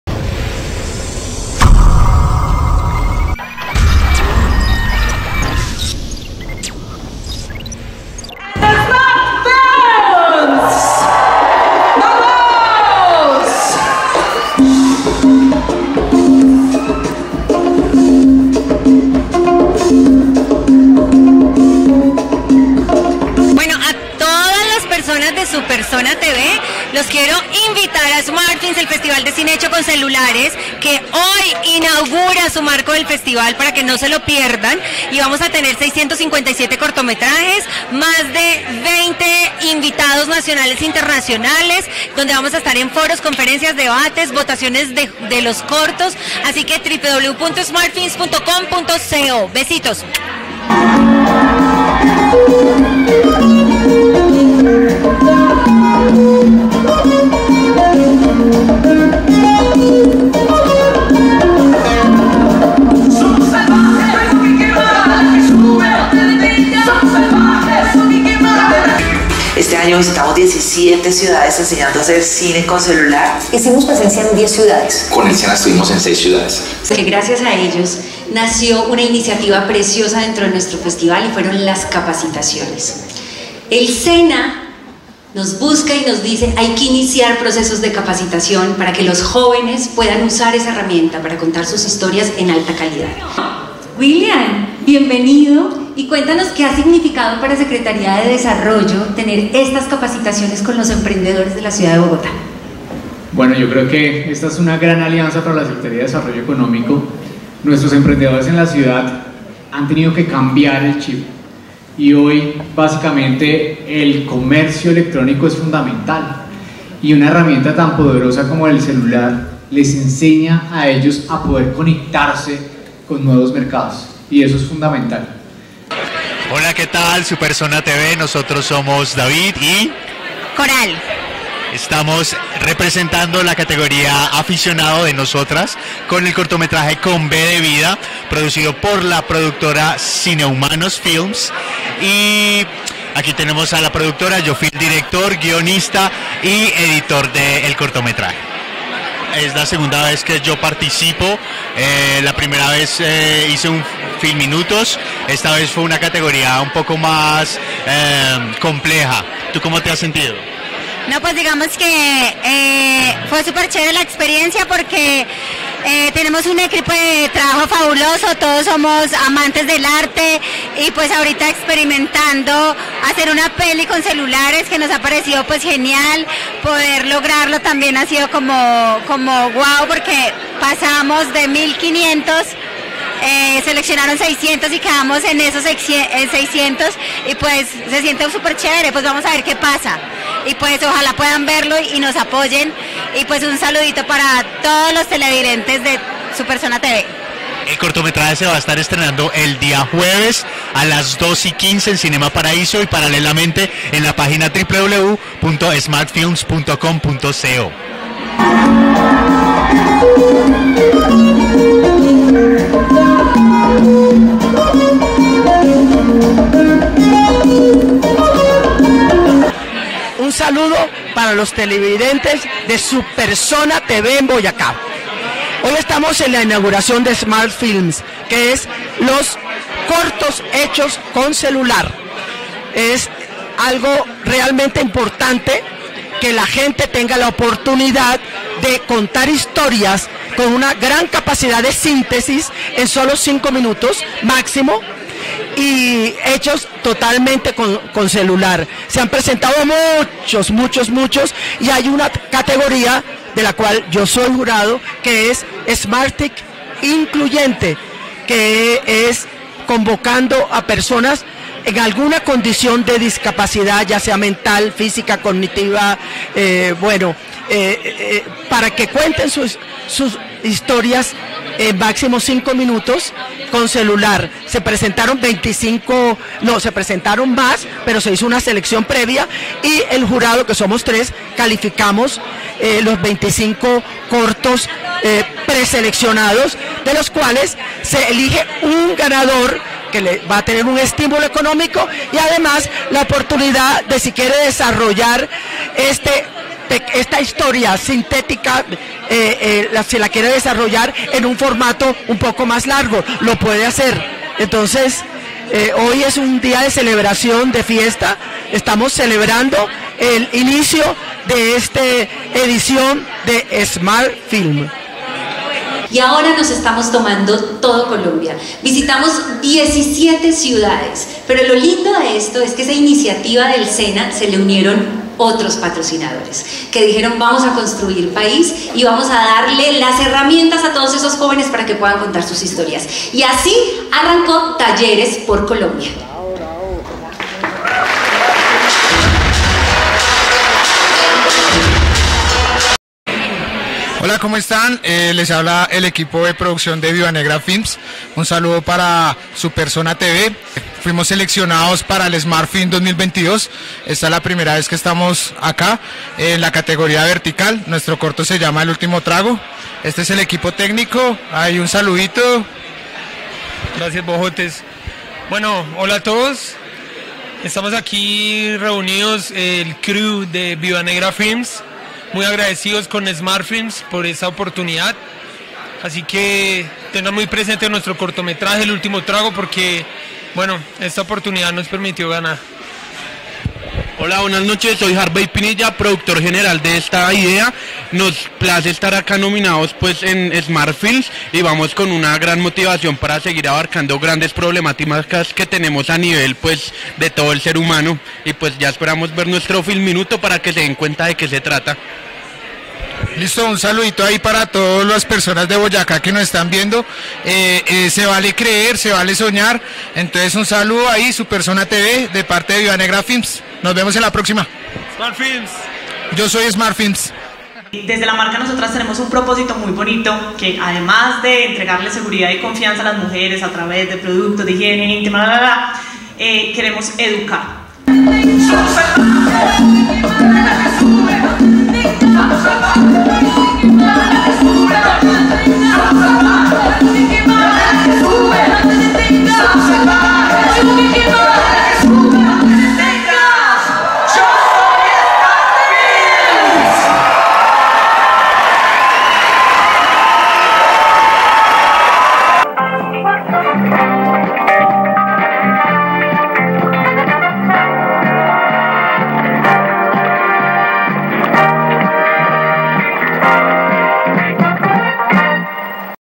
Smart Films 2022 - Lanzamiento.
Del 6 al 10 de Septiembre se realiza en Bogotá este importante Festival de Cine realizado con Celulares...